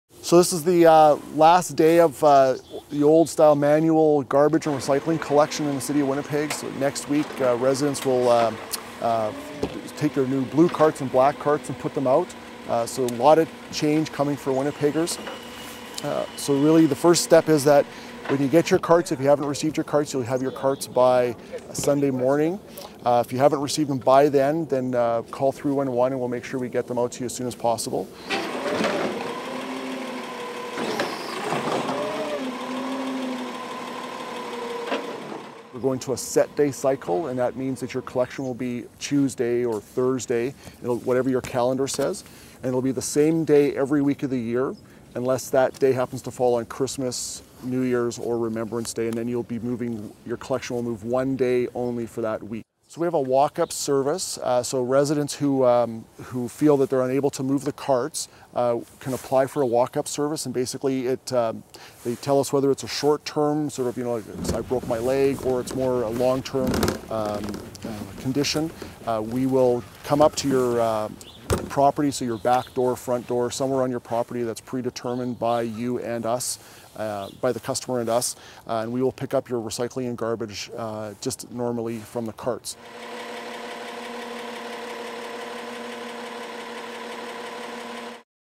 Winnipeg's Automated Recycling and Garbage Collection - September 28, 2012 - Winnipeg, Manitoba
The City of Winnipeg's new automated recycling and garbage carts are demonstrated as the program rolls out city-wide.